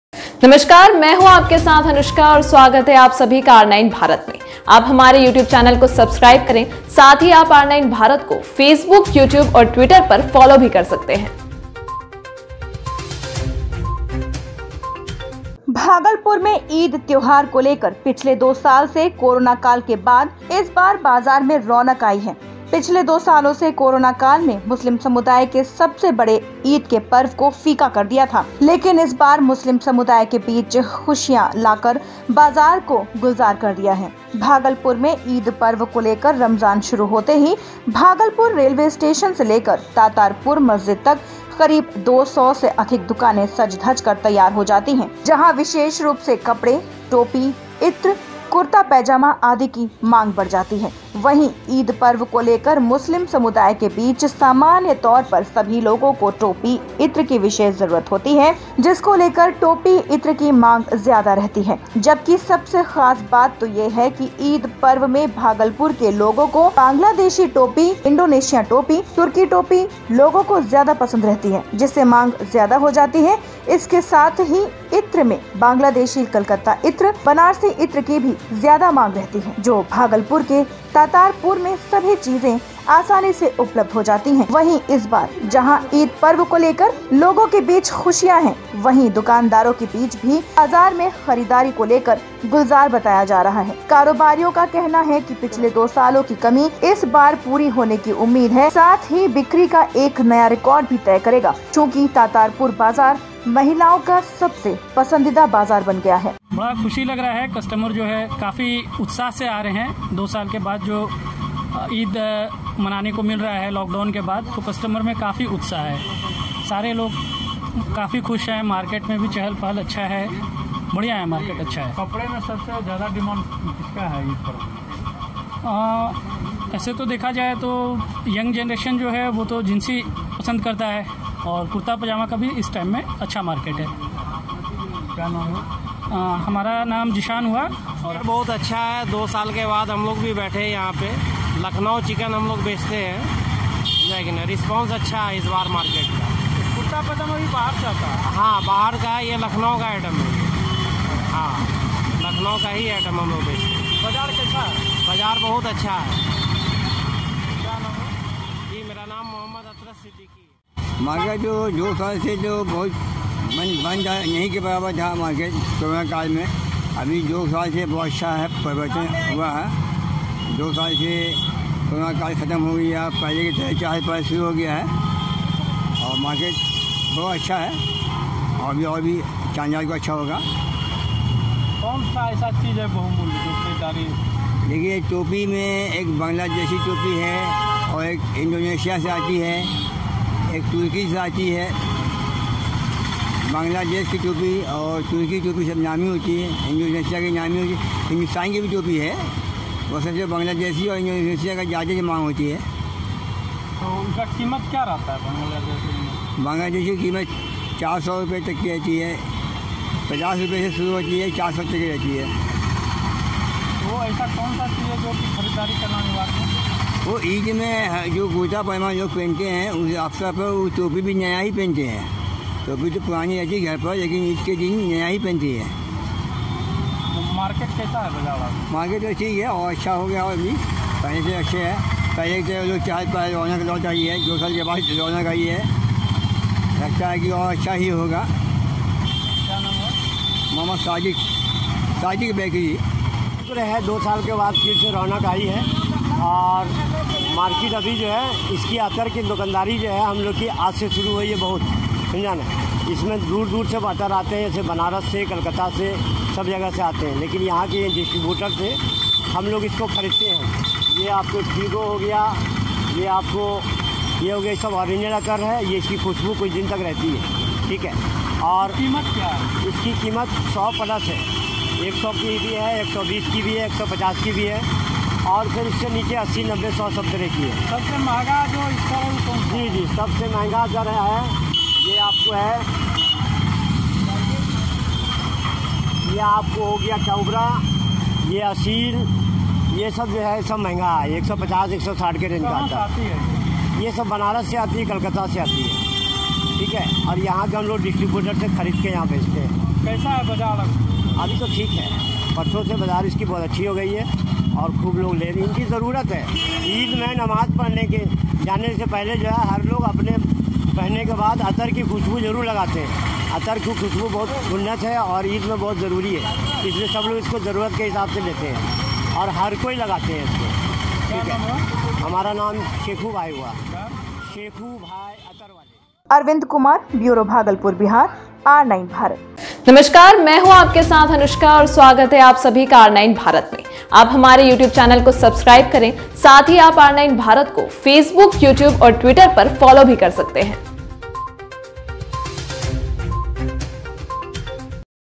भागलपुर में ईद त्योहार को लेकर पिछले दो साल के बाद बाजार में आई रौनक
अरबिंद कुमार भागलपुर